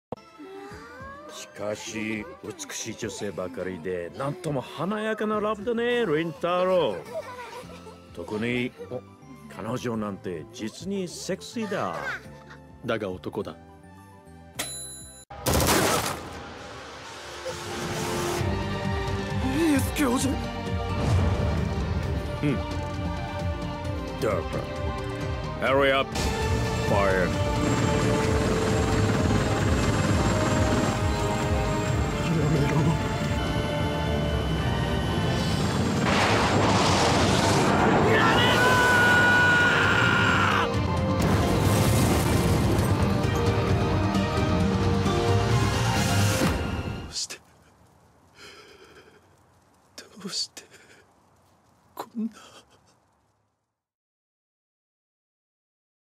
The REAL Reason Why Leskinen Turned Mad
After countless days of doing research on Professor Leskinen, this is what I came up with. The undeniable facts and the unspoken truth.